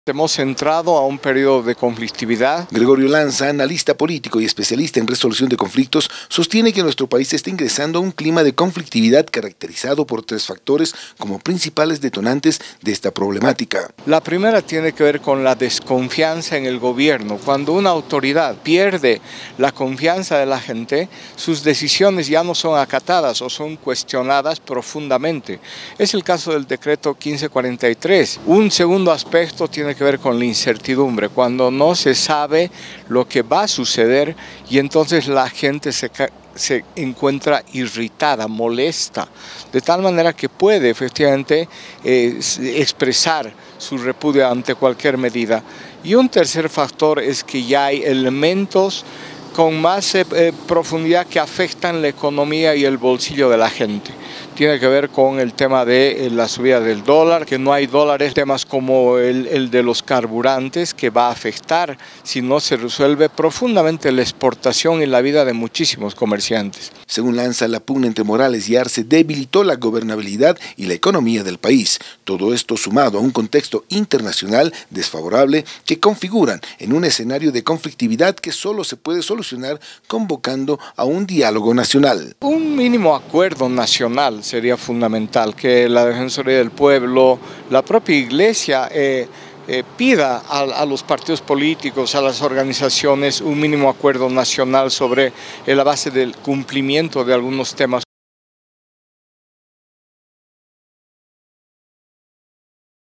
Desconfianza, incertidumbre y crisis económica
Tres factores avivan el clima de conflictividad en el país. Las pugnas internas y el contexto internacional desfavorable acrecientan esta problemática.